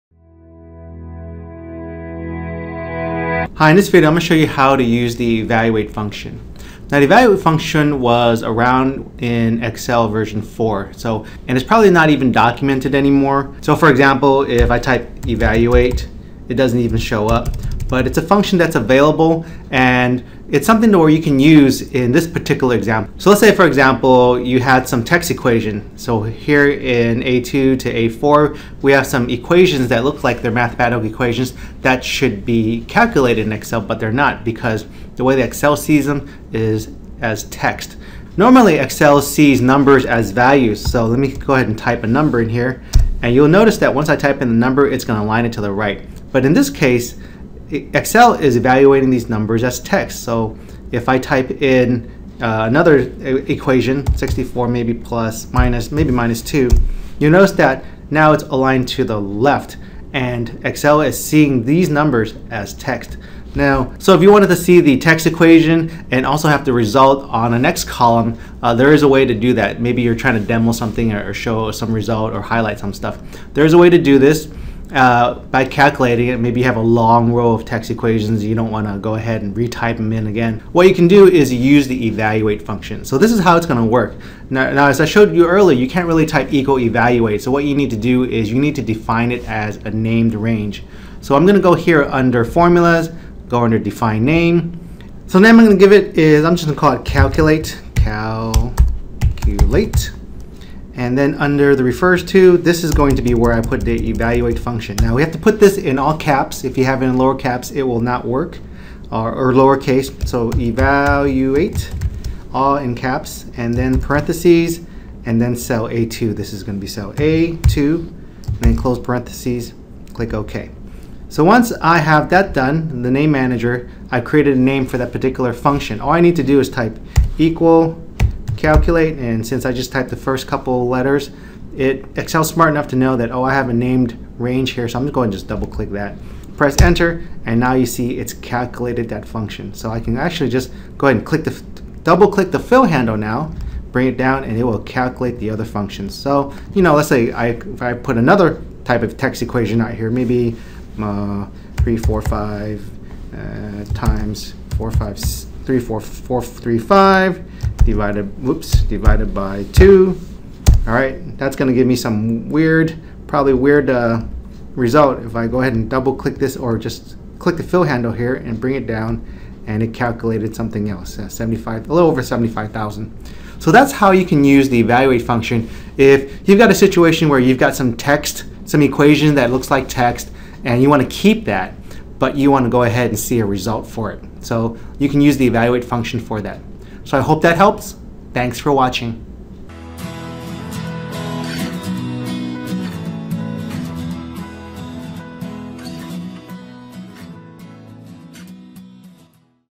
Evaluate a Text Equation
This video shows how to evaluate or calculate an equation that is represented as a text value.  Excel usually aligns numbers to the right of the cell and text to the left.  In this case the "numbers" in the equation are aligned left so Excel is representing them as text. To keep the view of the text equation but also see the calculated result in another cell we can use an old Excel function called EVALUATE to do this.